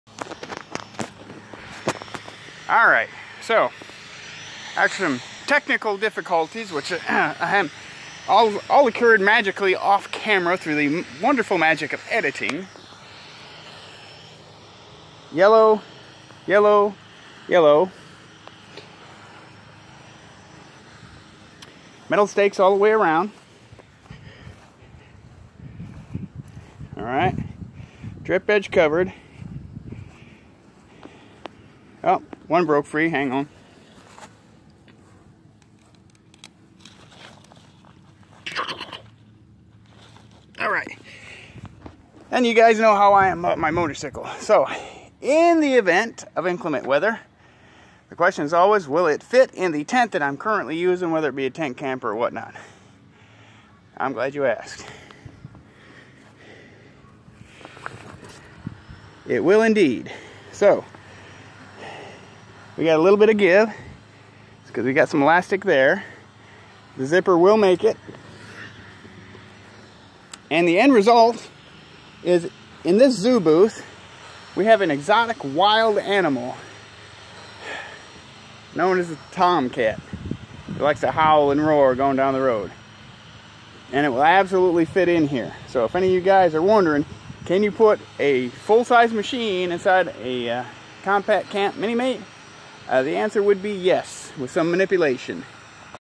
Set up Mini Mate with a surprise screen room guest
I set up mu Mini Mate in the front yard of the Fireman's place yesterday. I got it mostly wrong, but gained hood experience. Thinking on it later yesterday I realized how I got it wrong, so the second time is much less painful.